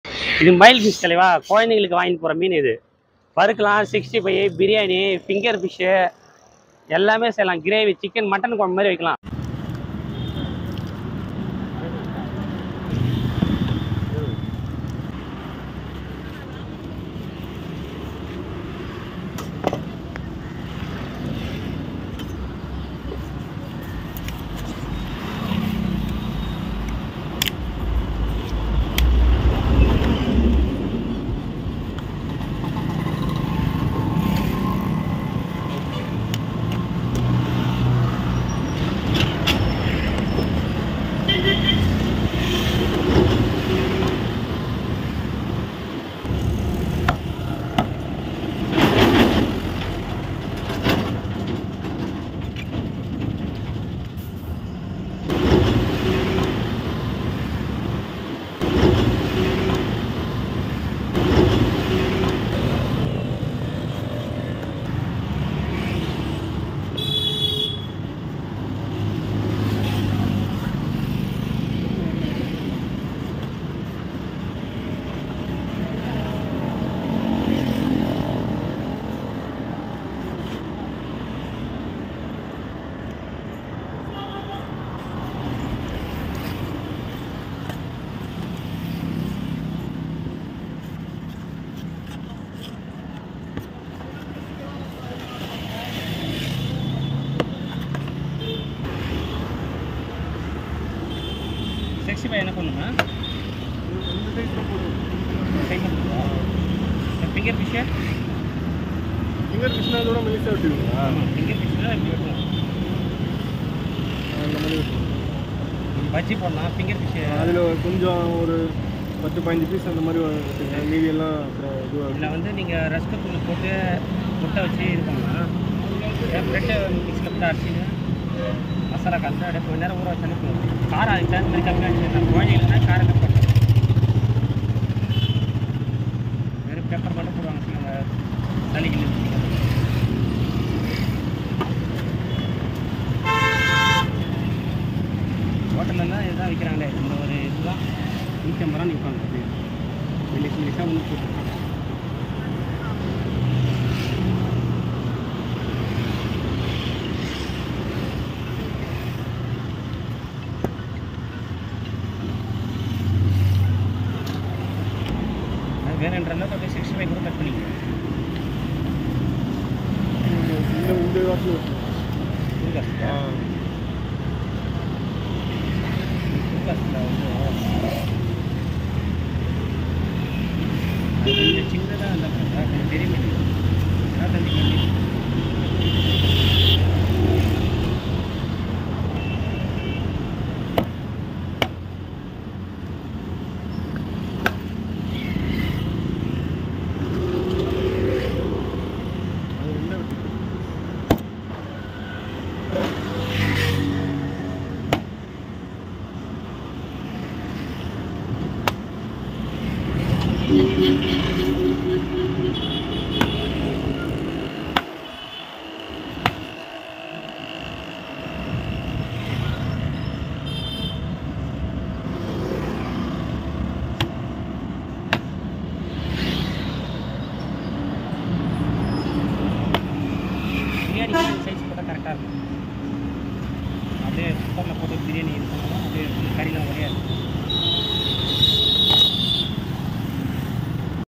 30 Kg Mayil Fish Best For 65, Fish Biryani Selling On Road
30 Kg Mayil Fish Best For 65, Fish Biryani Selling On Road